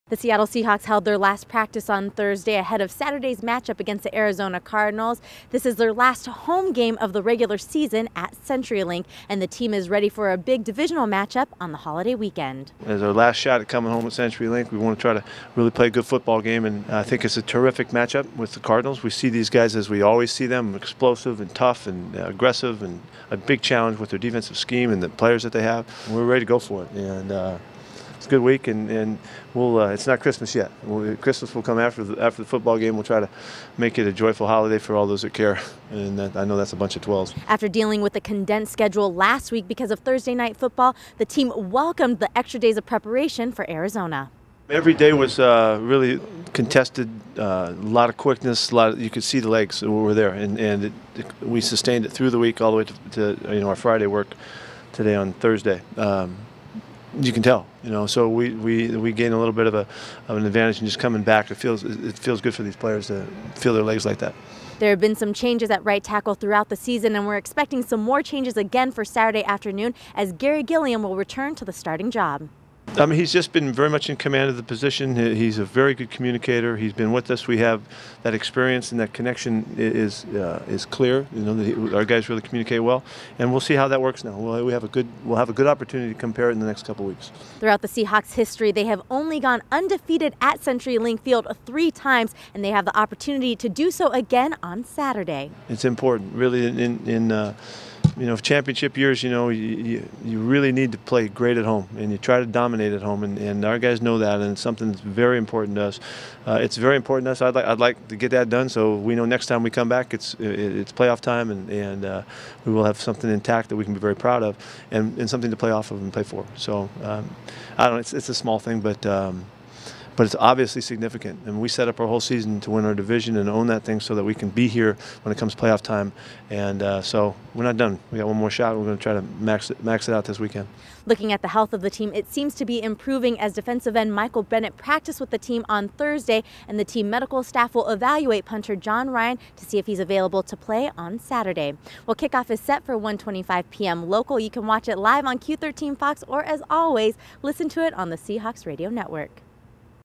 Friday Practice on a Thursday
Coach Pete Carroll gives an update on the most recent injury report as the Seahawks get ready to face the Cardinals at CenturyLink Field on Saturday.